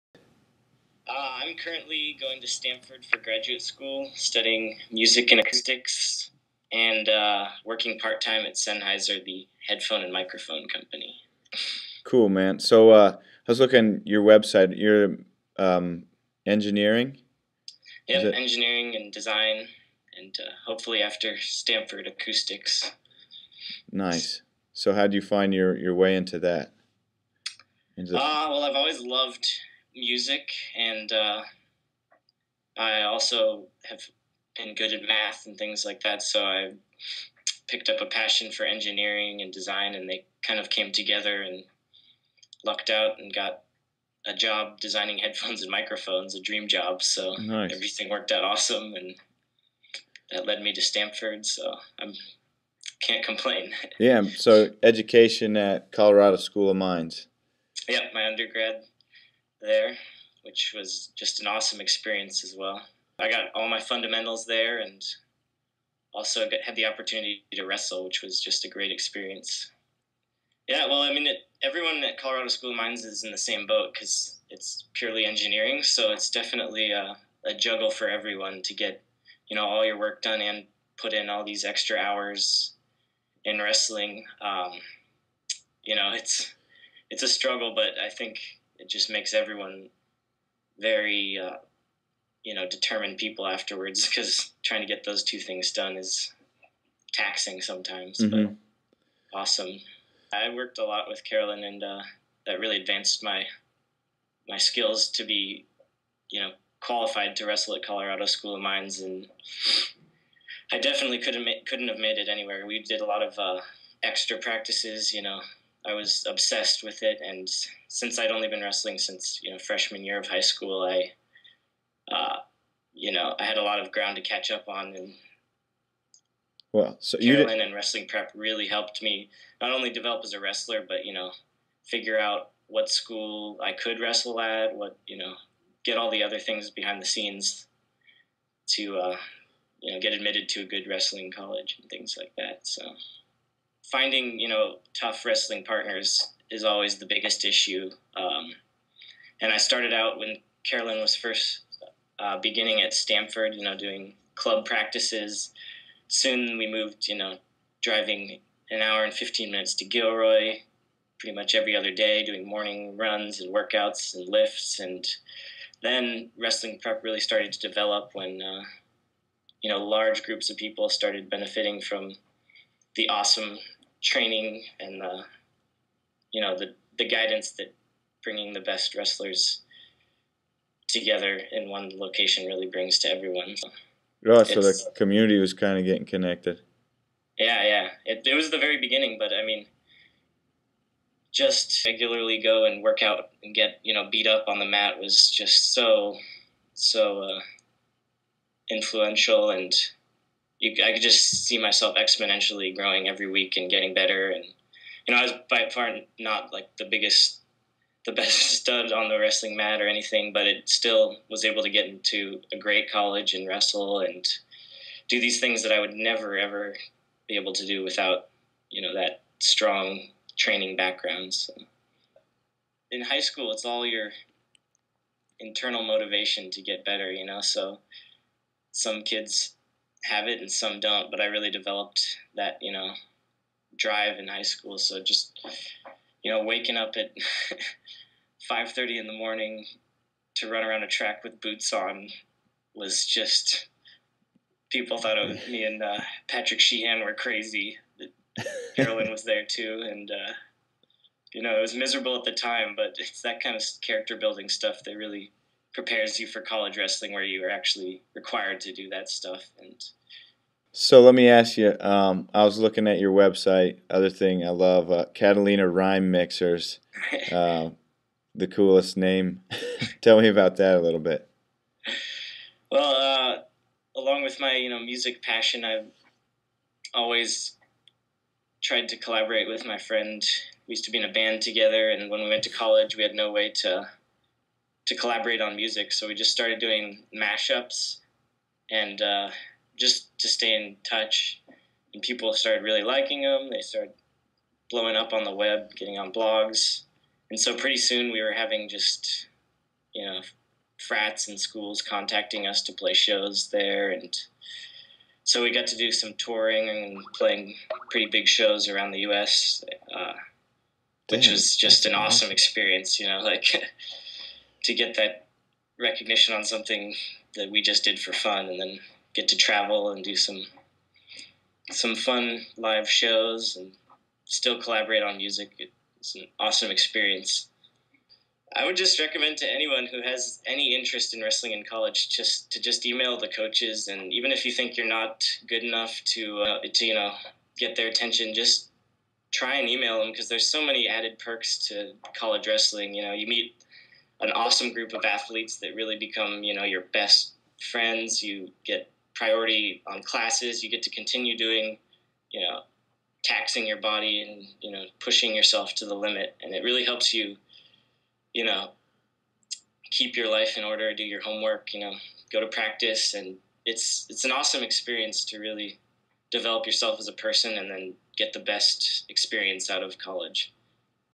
Cooper Newby: Engineer, Musician, Wrestler
Cooper Newby, Wrestling Prep Member, talks about how wrestling helped him develop as a student and in life.